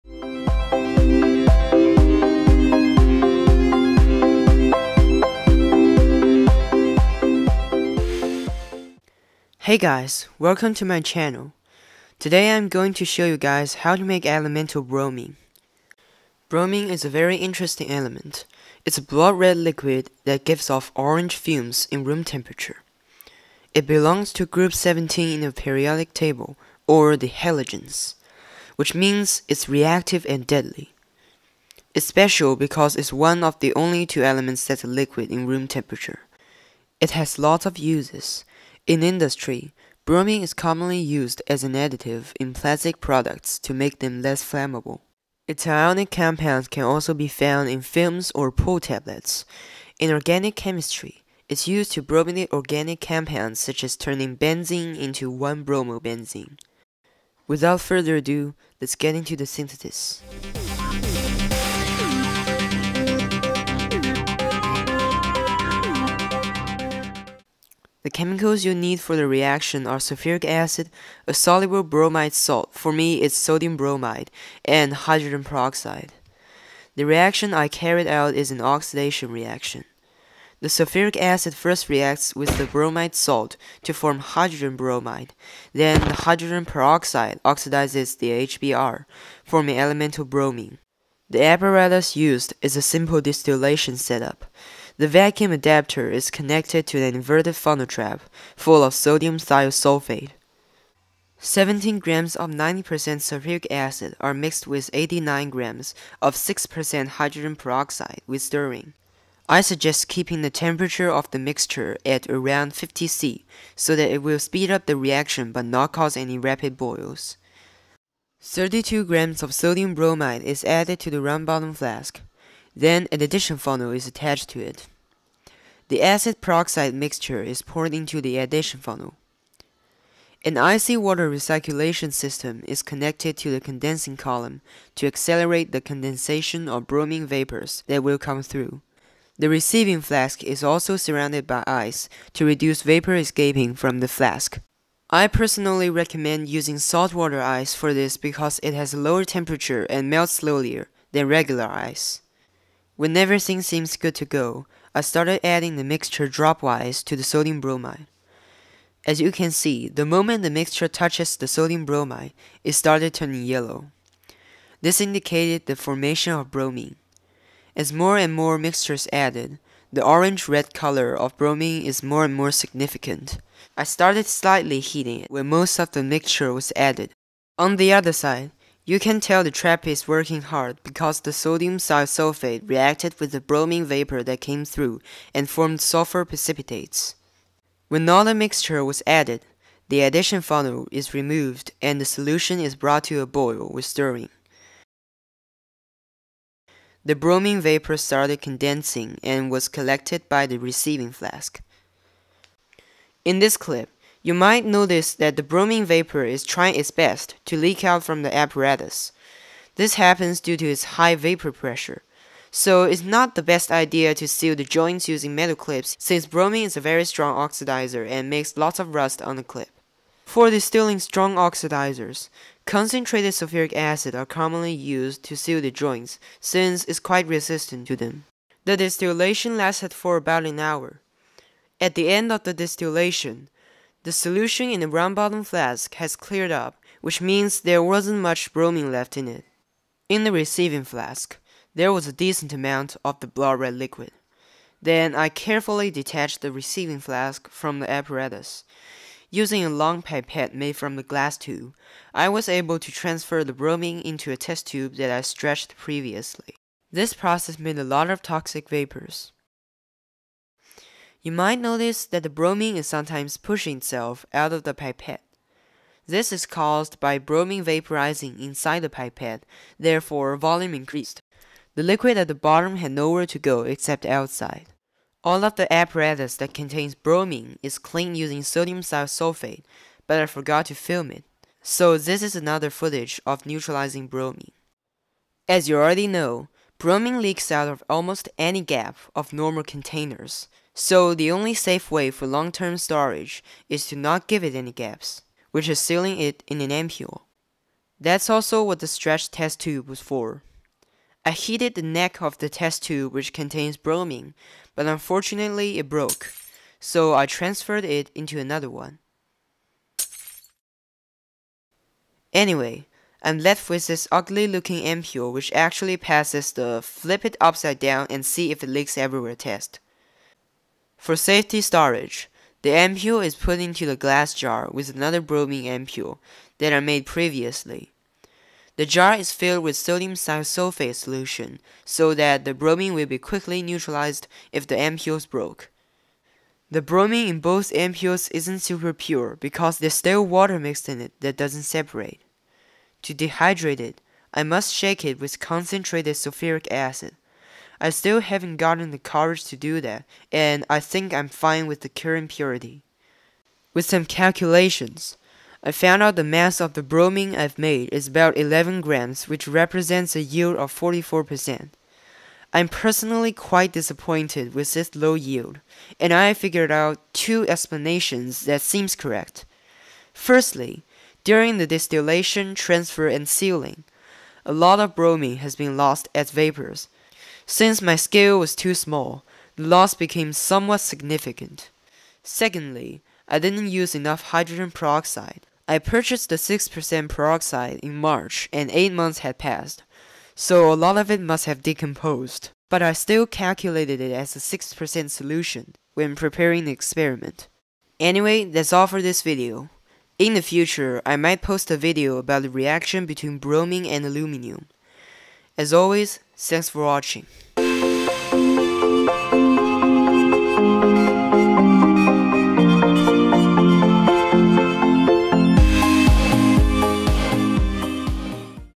Making Elemental Bromine
In this video, I'll be making bromine from the oxidation of hydrogen bromide. The hydrogen bromide is produced using sulfuric acid and sodium bromide.
The idea of this reaction comes from @NurdRage 's video about making iodine.
If I got anything wrong, I'm glad to hear your opinions in the comments.